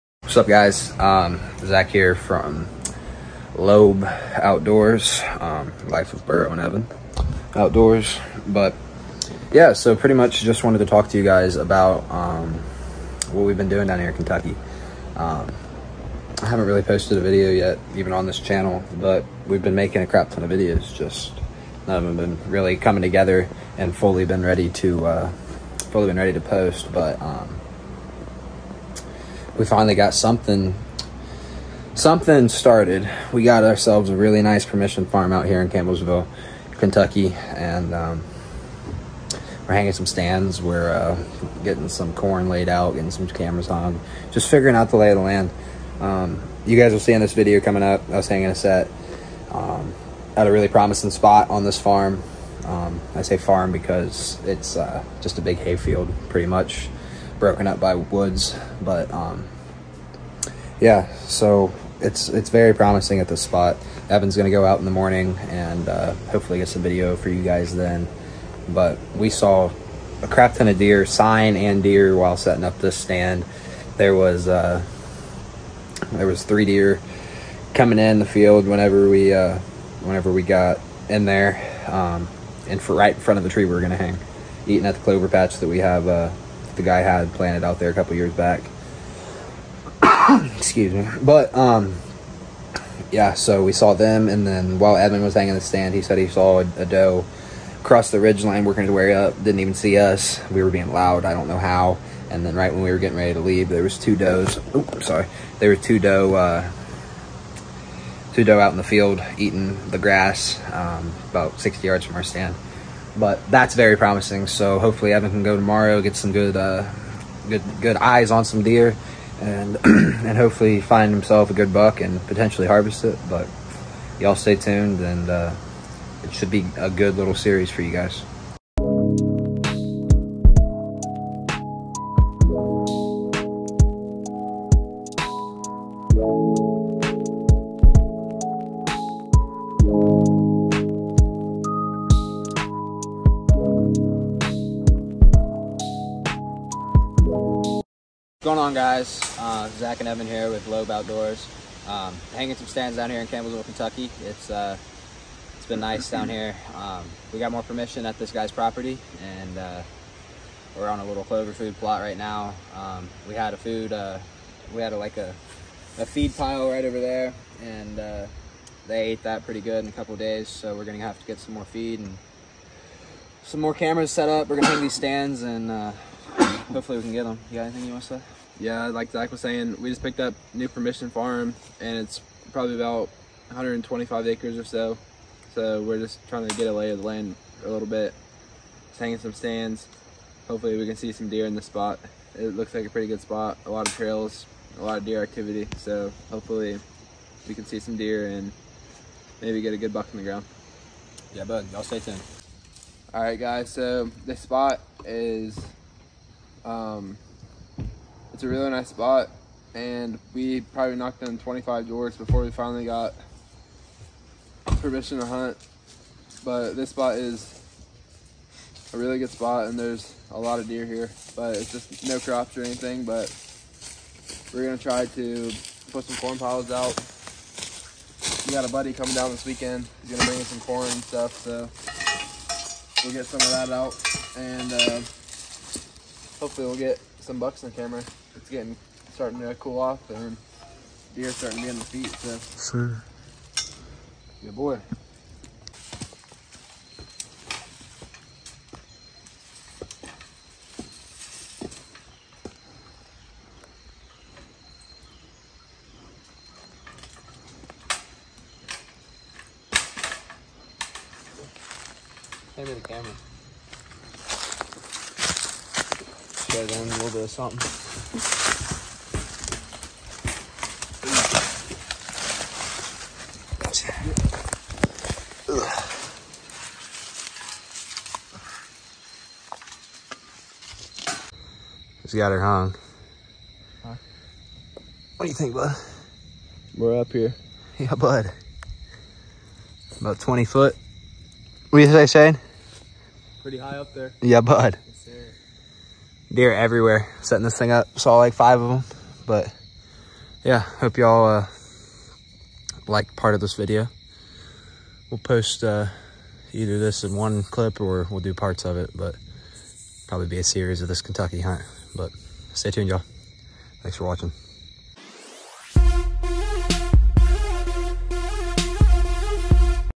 NEW LAND IN KENTUCKY!!!
Hanging our first stand at our new place down here in Campbellsville. Should be a great little series for you guys.